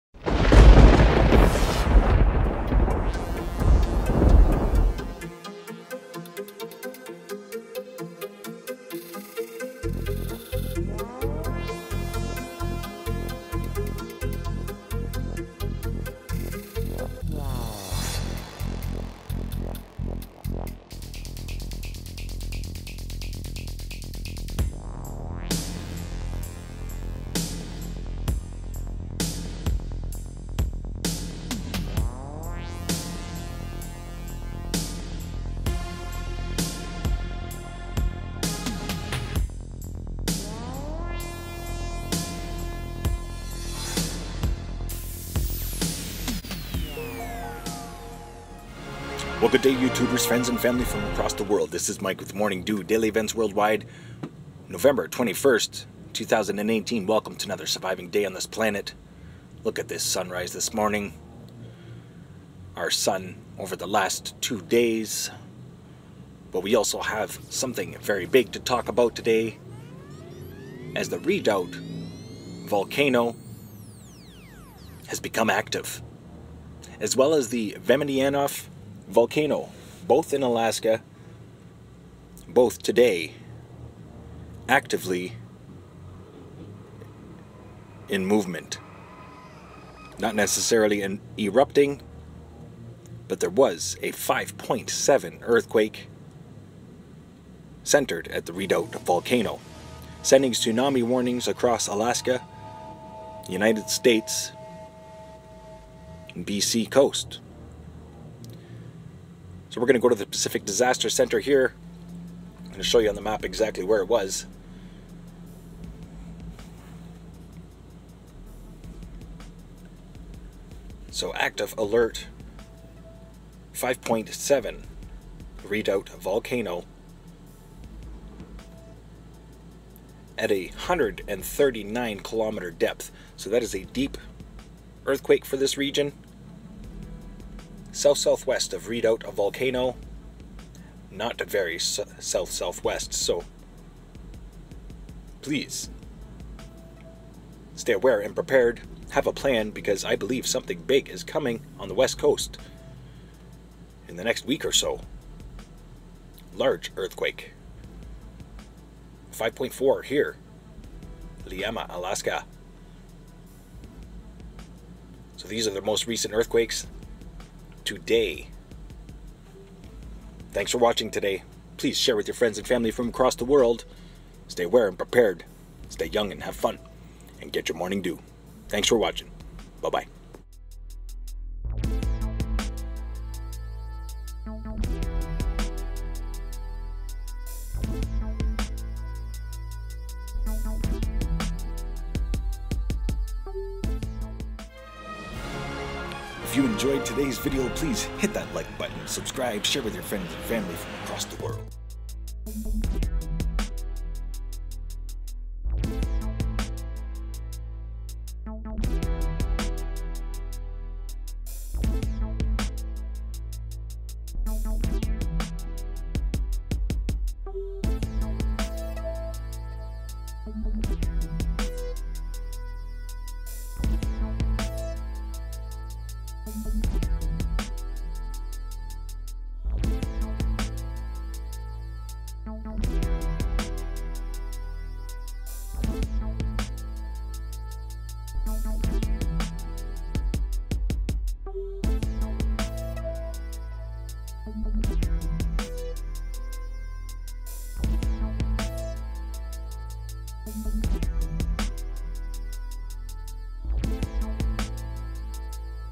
5.7 Earthquake Redoubt Volcano/ Tsunami warnings Issued November 21, 2018
-                                                Morning D.E.W.
Get Your Winter Gloves, Weather Stations, Basic Camoflauge SURVIVAL packs And other things... Drones, cameras etc.